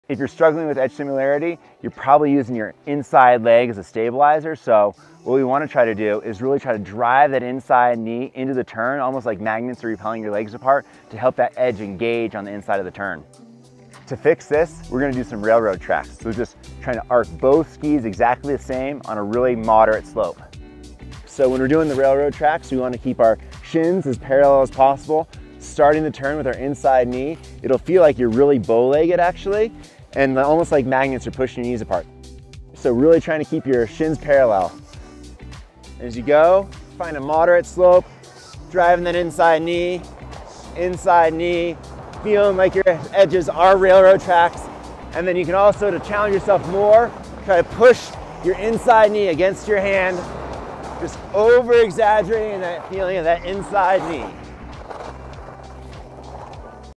Ski Tips From An Olympian!
Who better to take tips from than two-time Olympic gold medalist - Ted Ligety 🥇

Get Carv | Digital Ski Coach | Ski Tips | Real-time Feedback